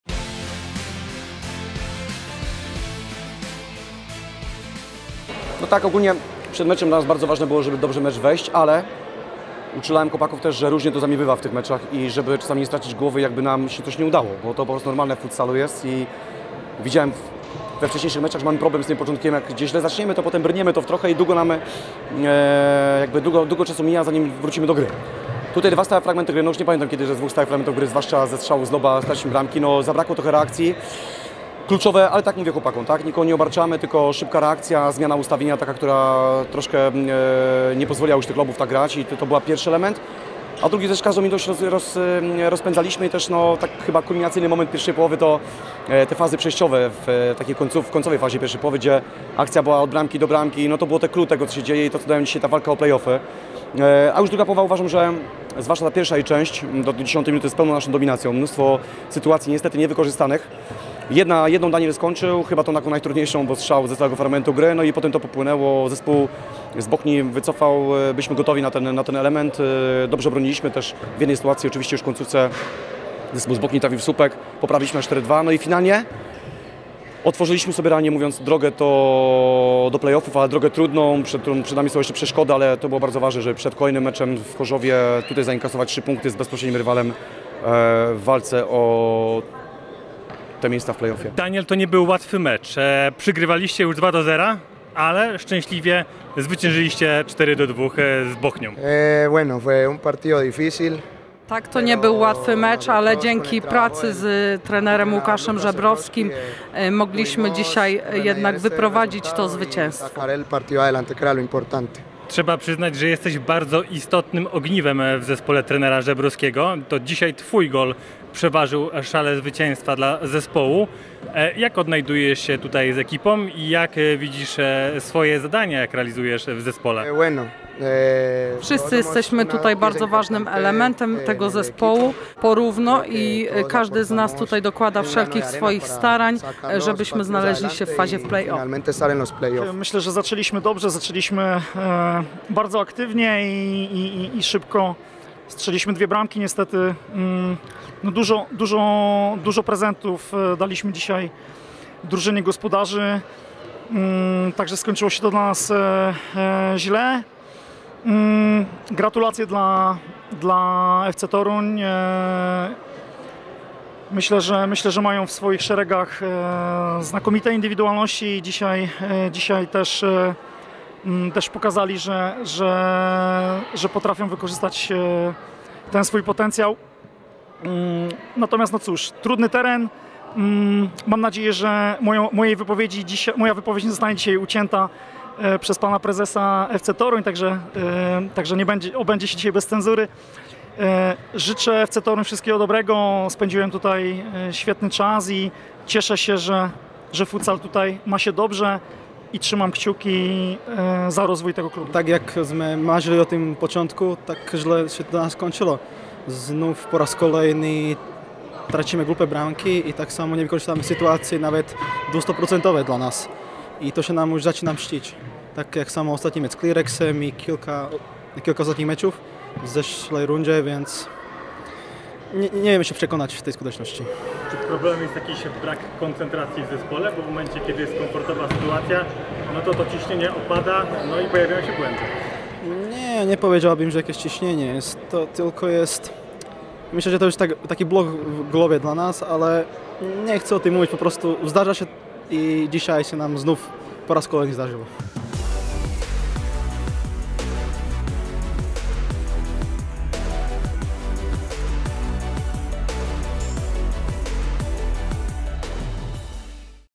FC Reiter Toruń - BSF ABJ Bochnia 4:2 | WYPOWIEDZI | 21. kolejka (2022/23)
FC Reiter Toruń - BSF ABJ Bochnia 4:2 (2:2) | 21. kolejka - sezon 2022/23

BRAMKI: Krzysztof Elsner 8, 39, Daniel Gallego 14, 26 - Piotr Matras 2, Adam Wędzony 7.

Po meczu wypowiedzieli się trenerzy Łukasz Żebrowski i Klaudiusz Hirsch oraz zawodnicy Daniel Gallego Garcia i Kevin Kollar.

Oglądaj MECZE KOLEJKI w każdy poniedziałek o godzinie 18:15 w TVP Sport!